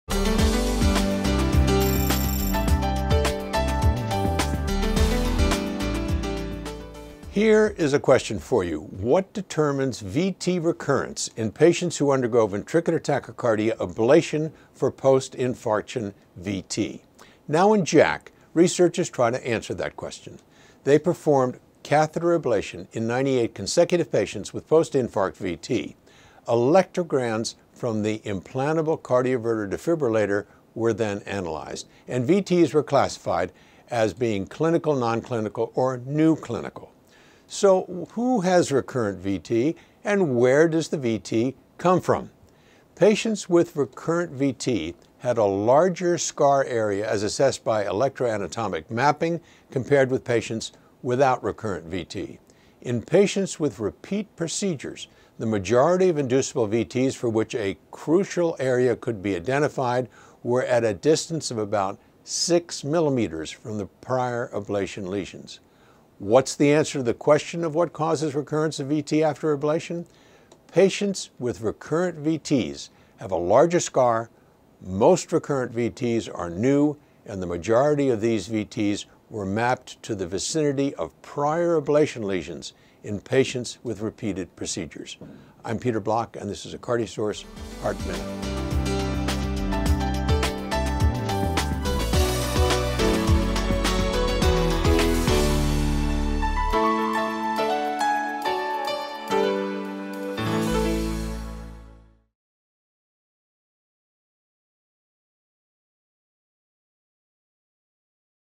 Heart Minute | Causes of Recurrent VT after Ablation
What determines VT recurrence in patients who undergo ventricular tachycardia ablation for post-infarction VT?